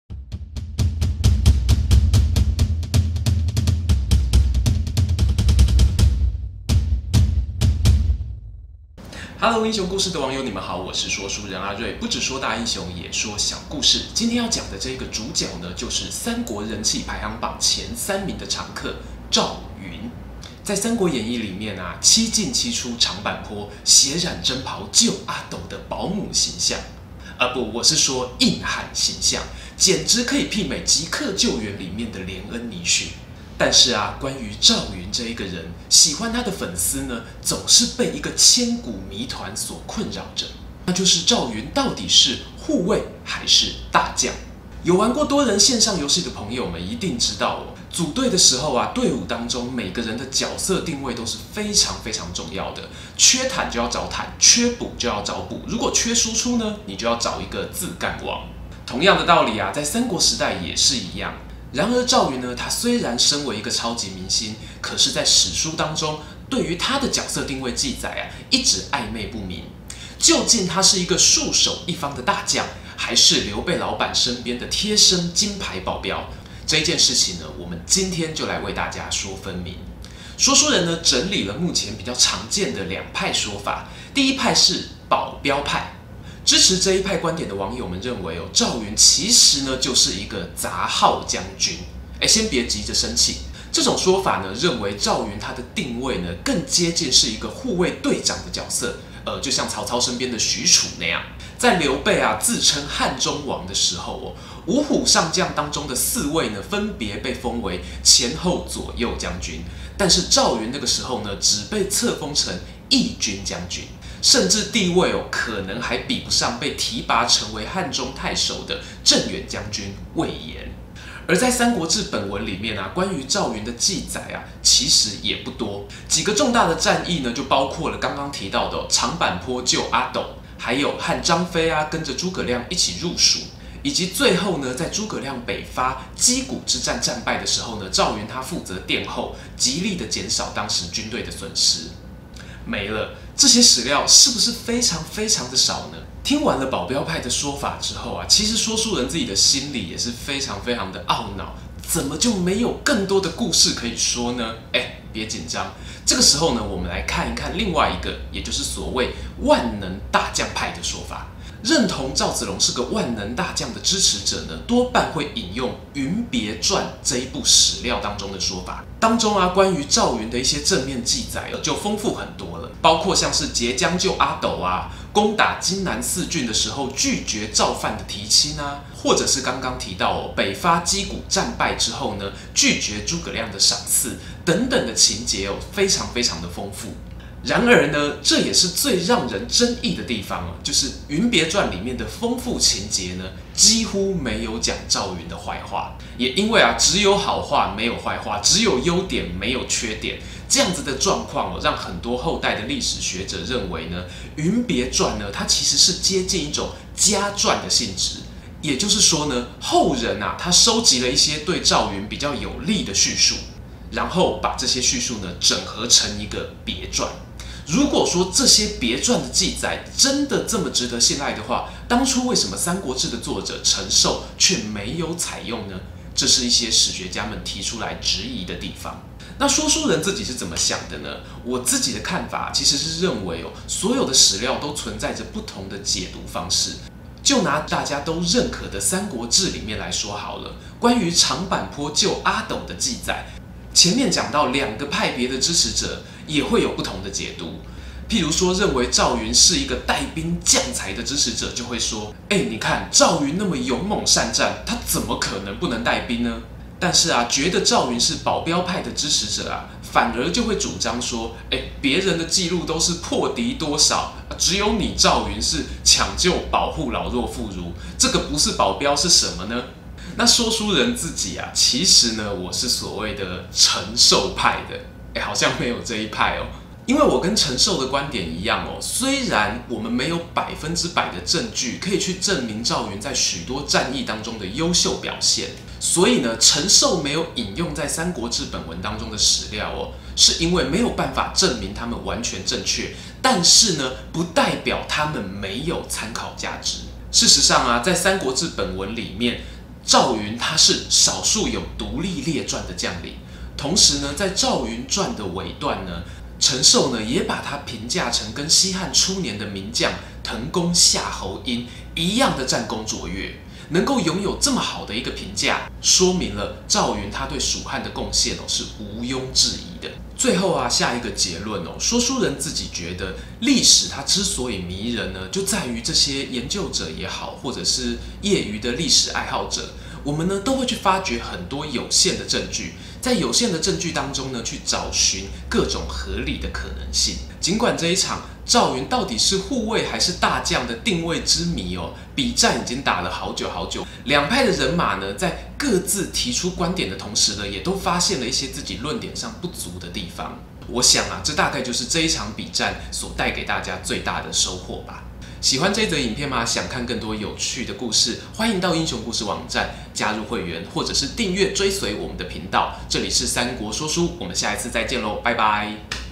趙雲是領軍的大將？還是只是劉備的親衛隊長？｜【三國說書】#7
白馬銀槍、當陽救主的常山趙子龍，是少數在後世評價接近「人格完美」形象的三國人物。

正所謂：人紅討論多。針對趙雲人品部分的爭議較少，但關於他在蜀漢作戰上的定位，就有很多熱烈的辯論，譬如：他到底是護衛還是大將？請聽本日的三國說書～

編劇：阿睿
剪輯：Mei

「你所喜歡的戰爭與冒險，你想看的英雄故事都在這裡。」